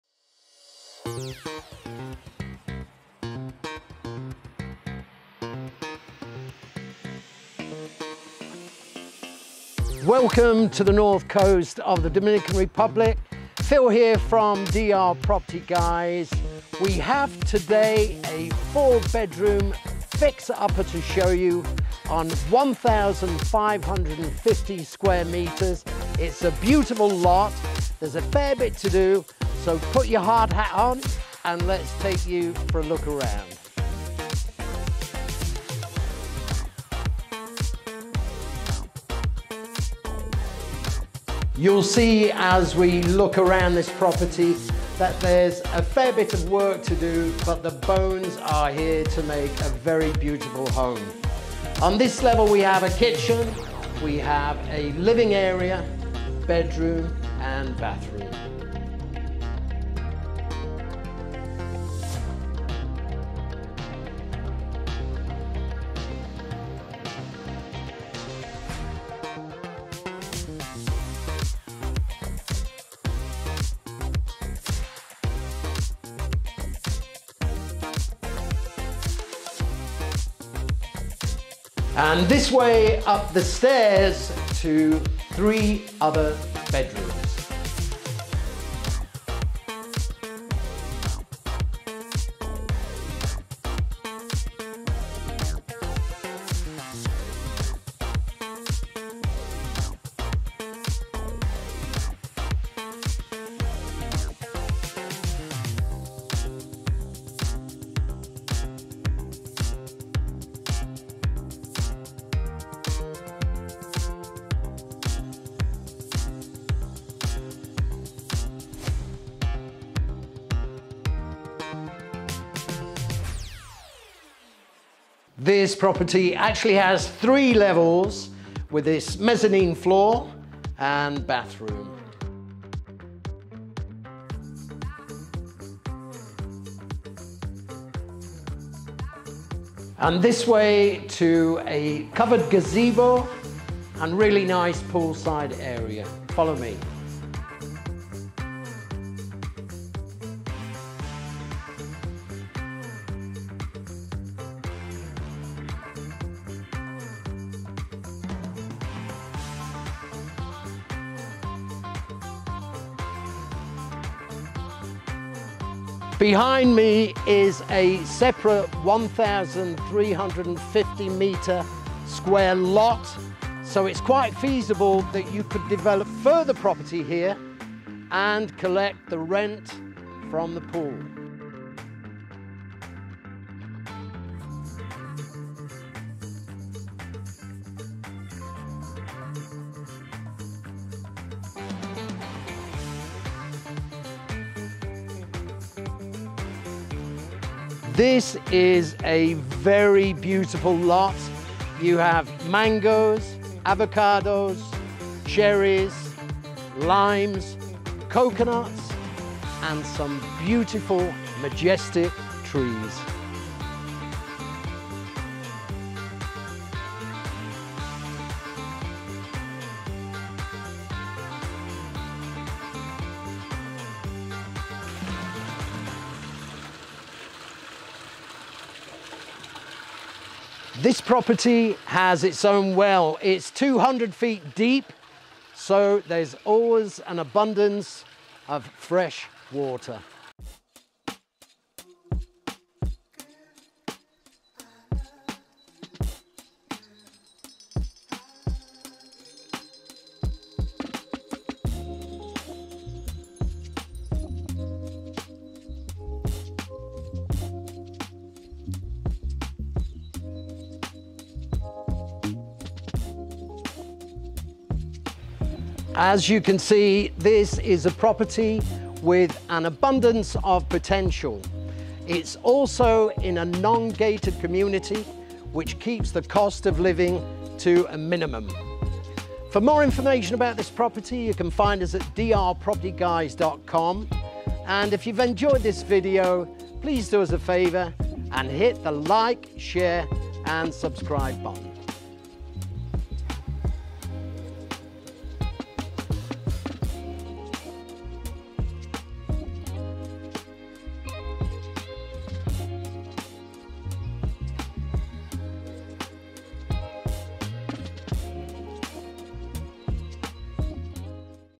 Fixer Upper In The Hills
Set on 1,527.01m2 of prime mature, hillside land, this 4 bedroom, 3 bathroom home really has some character to it.  Upon entering the fully walled and fenced property, you can immediately appreciate the grandure of this once majestic home, a plethora of mature trees including mango, avocado, lime, cherry and coconut amongst others are some of the elements that greet you upon entry.  The gate, which could be turned automatic, leads to parking space for at least 2 vehicles.

Upon entering the house itself, the first thing that hits you is the open and bright living area, that leads invitingly to the outdoor gazebo and pool area.  The kitchen and master bedroom with en-suite are also located on the ground floor.  The mature trees once again play their part in enticing you to sit in the living room and stare at them for hours, the open side of the living room really creates a feeling of tropical paradise.

There is a stairway up to the 1st floor, on which level you will find 3 further bedrooms, one of which has an en-suite bathroom, the other 2 share a bathroom.  There is a further mezannine with it's own bathroom.

Once we walk out of the main living room to the gazebo, we once again find ourselves in a tropical dream.  The swimming pool is in perfect condition and has it's own swim up bar.  This property also has it's own water well, drilled to some 200ft of depth.

The total construction size on this property is around 200m2.  There is an adjoining lot of 1,366m2 which is sold separately for US$95,620 but if the buyer wishes to purchase both this home and the lot next door, a better price can be agreed upon.

Now this property certainly is a project, something that will require a good chunk of cash to be invested, however once the works are done you will be looking at a stand alone property with some features that many others on the market do not have.

Price: U$295,000
Lot size: 1,527.01 m²
Construction: 200 m²

Adjoining Lot: 1,296.1m², price U$85,000 – Discounted price for both properties sold together U$350,000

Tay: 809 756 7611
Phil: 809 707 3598

If you’re in the area, please let us know, it would be our pleasure to show you around and share with you our combined 50 years of living here in The Dominican Republic.